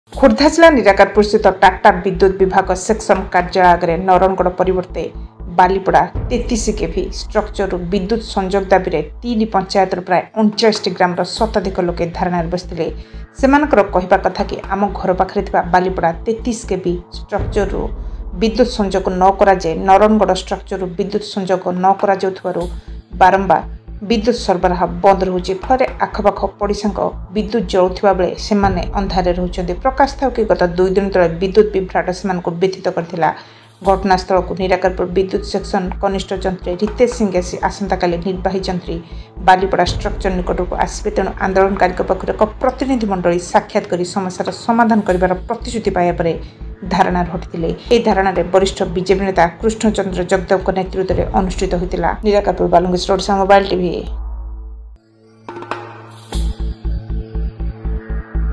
ନିରାକାରପୁରର ବିଦ୍ୟୁତ କାର୍ଯ୍ୟାଳୟ ଆଗରେ ଧାରଣାରେ ବସିଲେ ୩୯ଟି ଗ୍ରାମର ଲୋକେodishamobiletv
ସାରା ଦୁନିଆଁର ଟିକ୍ନିକ୍ ଖବରକୁ ନେଇ ଭିନ୍ନଢଙ୍ଗରେ ପହଞ୍ଚାଇବାର ପ୍ରୟାସ,ନିରାକାରପୁରର ବିଦ୍ୟୁତ କାର୍ଯ୍ୟାଳୟ ଆଗରେ ଧାରଣାରେ ବସିଲେ ୩୯ଟି ଗ୍ରାମର ଲୋକେodishamobiletv